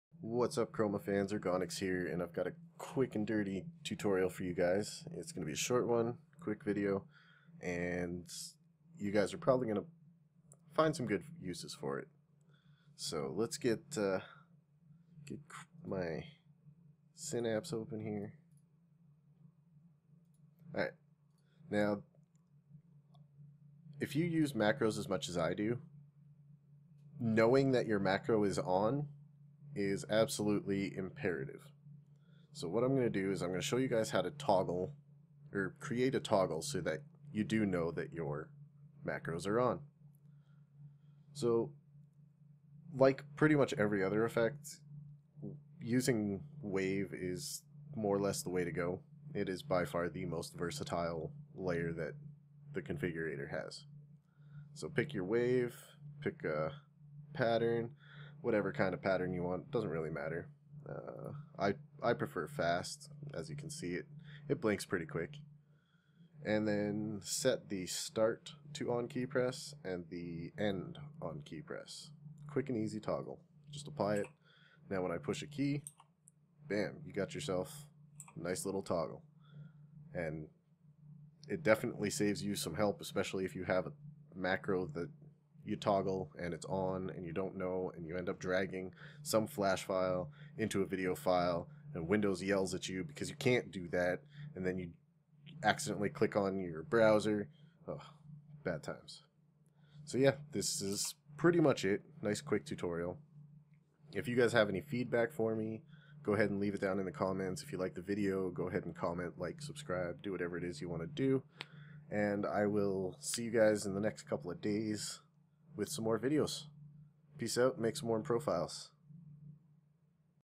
Blackwidow Chroma Configurator Tutorial - Toggles
In this quick tutorial, Ergonyx shows you how to use the Chroma Configurator to make toggled effects on keys for on/off states, something especially handy when it comes to macros.  Who knows what else you could use them for!